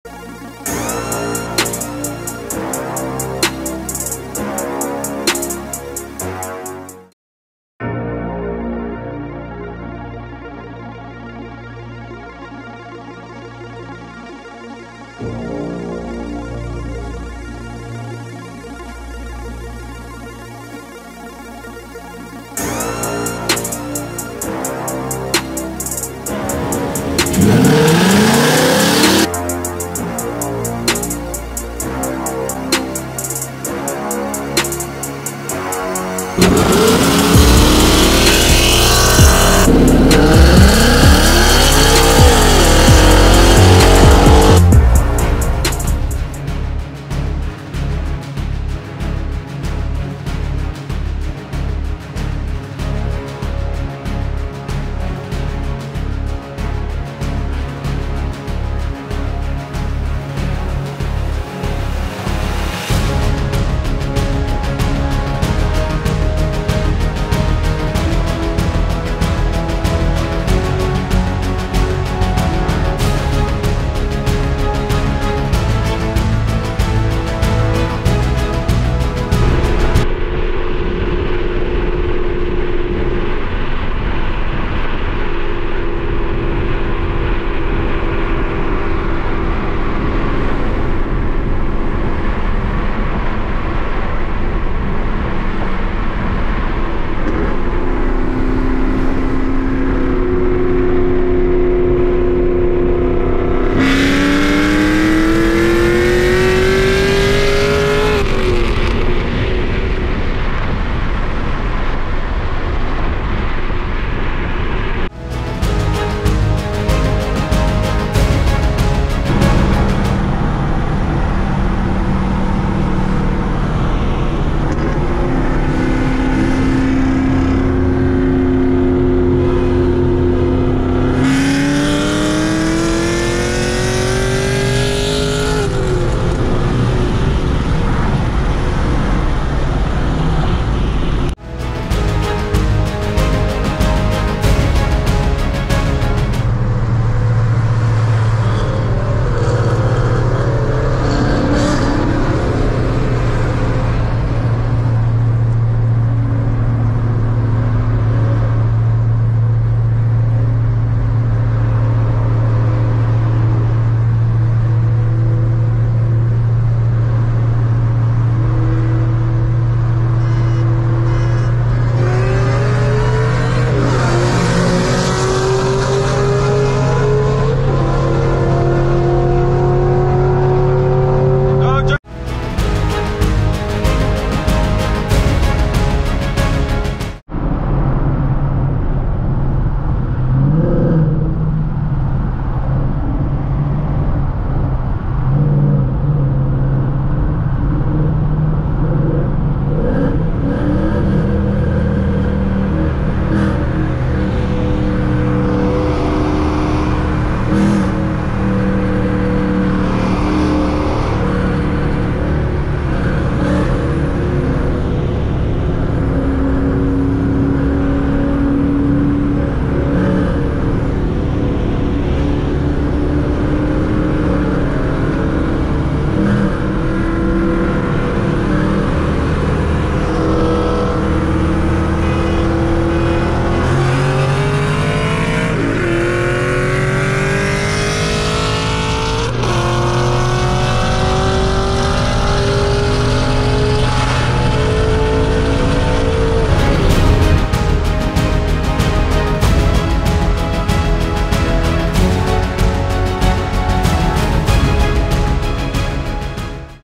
MUSTANG GT 5.0 10R80 E85 VS SVT COBRA TERMINATOR
MUSTANG GT - FBO, E85, SUSPENSION, 10R80, TUNE, 3.15 GEARS

MUSTANG COBRA -  POWER - 540/558
FBO/ported blower/E85

ALL RACES FILMED IN MEXICO